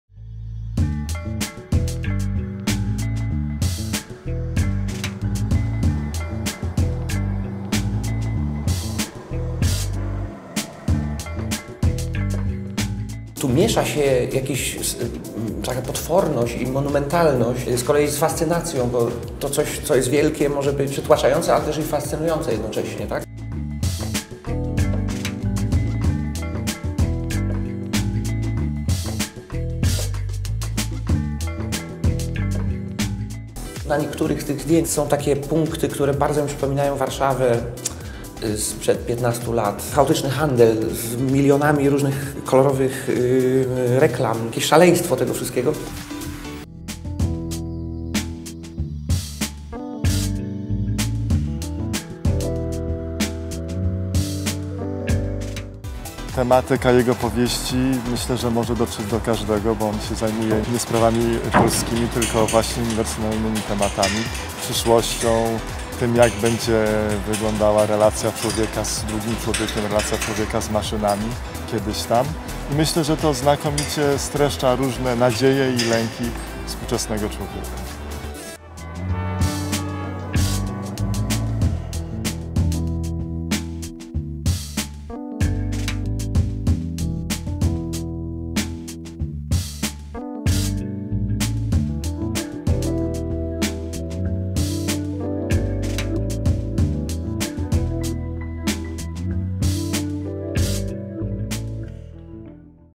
Presidency Videoblog: Cultural events in Moscow / Wydarzenia kulturalne w Moskwie
Wydarzenia programu kulturalnego polskiej prezydencji w Moskwie.

Events of the Polish Preisdency cultural programme in Moscow.